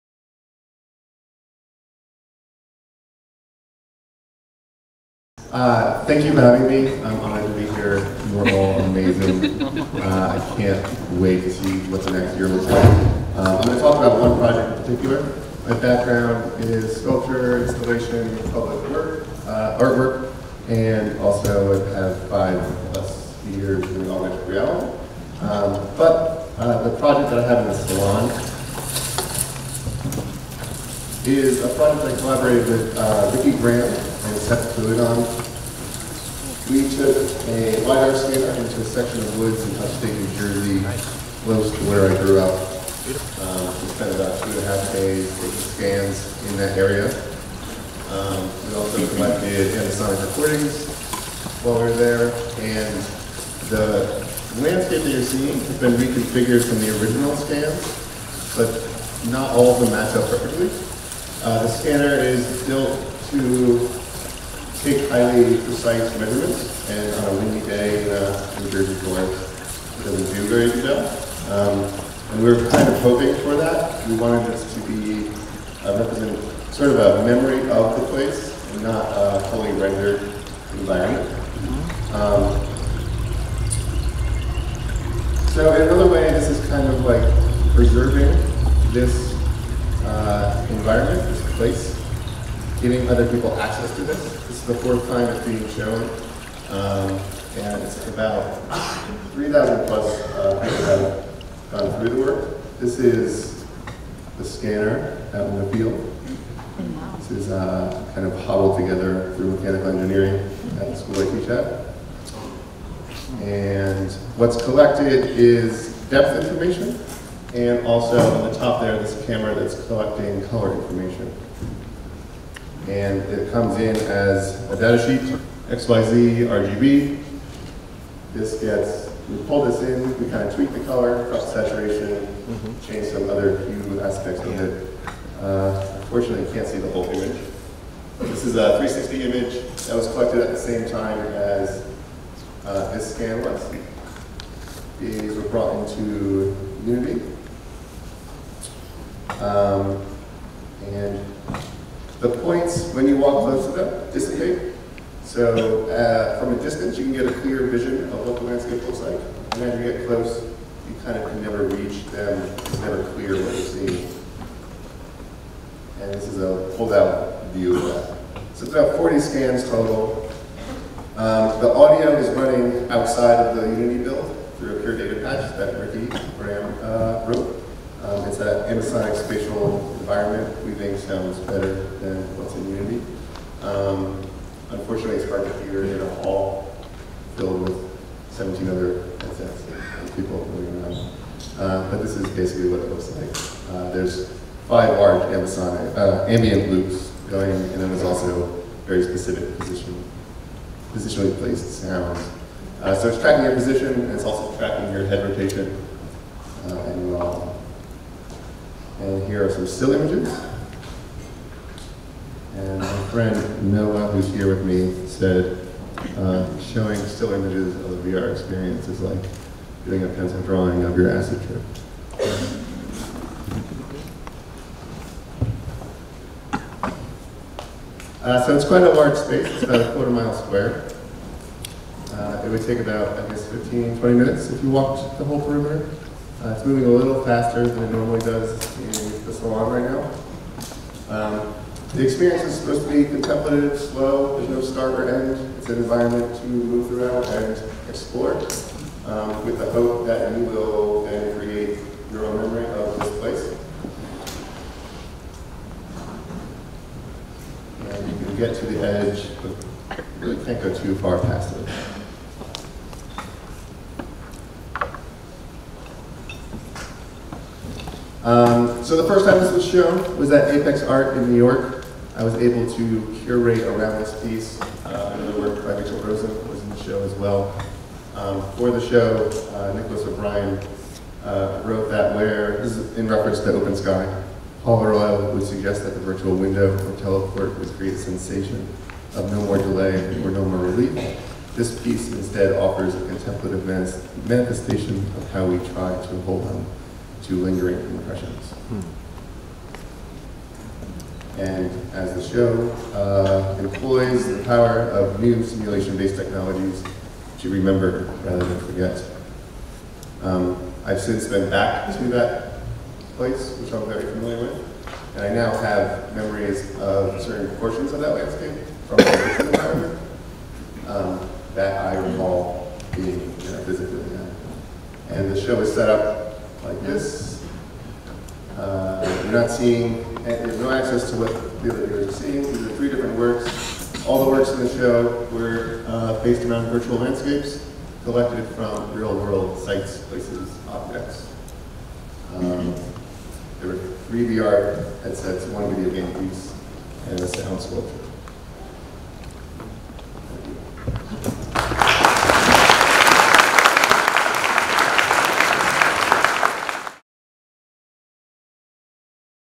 WEIRD REALITY: Head-Mounted Art && Code :: Christopher Manzione :: Speed Presentations
Christopher Manzione is an artist and curator whose work questions an object’s origin and presence in a world of ubiquitous digital and network technologies. He most recently curated
Space Between the Skies at New York’s Apexart. Manzione also pioneered mobile AR art platforms by founding the Virtual Public Art Project (2010), an organization that used augmented reality to produce original artist works in public space. Manzione was a founding member of Manifest.AR, an international artists’ collective working with emergent forms of augmented reality as interventionist public art.

He has shown nationally and internationally at venues such as the Boston ICA, Abington Arts Center, Philadelphia International Festival of the Arts, the Surry Hills Festival in Melbourne, and Gurzenich Koln Museum in Cologne. He is currently Assistant Professor
in Visual Arts and Technology at Stevens Institute of Technology.